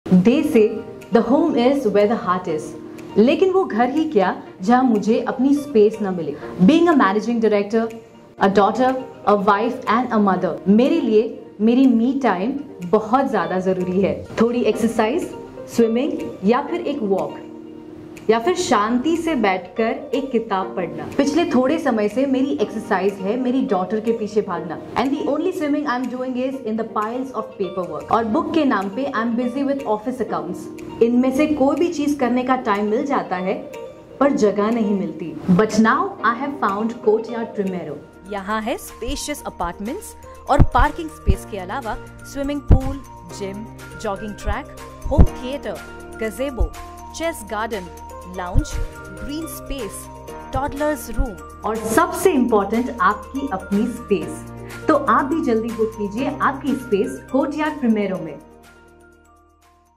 Courtyard Primero l Newly Launched Project l3 BHK Apartments Project l Vadodara
Lead a comfortable and luxurious life at Courtyard Primero, the place where all your real estate dreams come true. Here, establish the home you have always wanted for yourself without any hesitations.